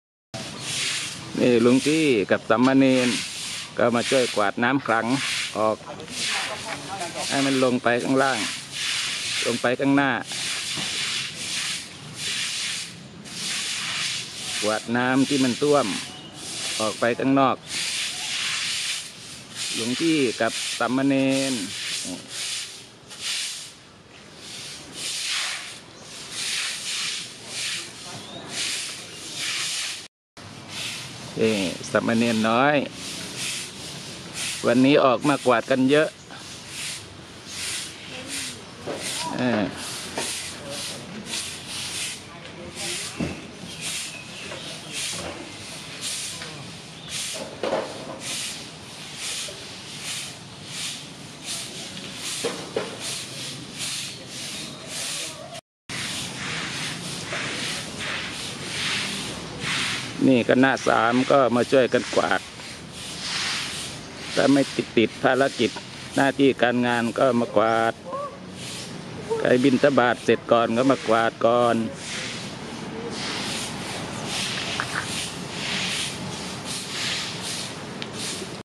หน้าที่ทุกเช้าต้องมากวาดใบไม้ดูแลความสะอาดภายในวัดเณรน้อยเจ้าปัญญา(เณรน้อยเจ้าปัญหา😁😁)
ภารกิจทุกเช้าต้องมากวาดใบไม้ดูแลความสะอาดภายในวัดฝึกตนขัดเกลาจิตใจลดความขี้เกียจเป็นหน้าที่ต้องปฏิบัติเมื่ออยู่กับวัด วัดกลาง พระอารามหลวง บุรีรัมย์